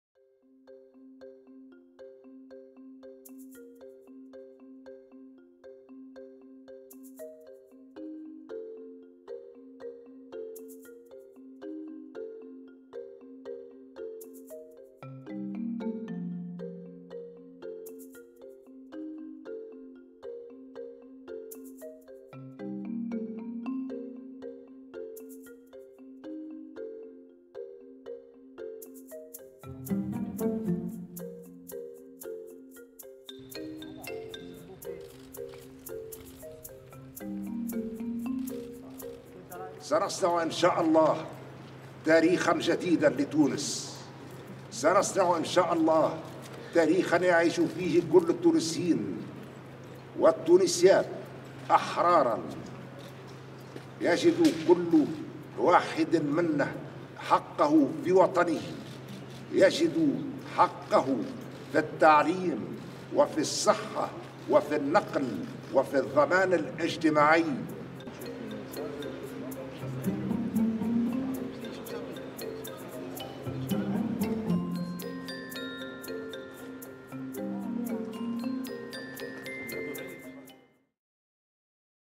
الرئيس التونسي يدلي بصوته في الانتخابات التشريعية | AFP
قال الرئيس التونسي قيس سعيّد عقب الإدلاء بصوته في الانتخابات التشريعية في تونس: "سنصنع إن شاء الله تاريخاً جديداً لتونس".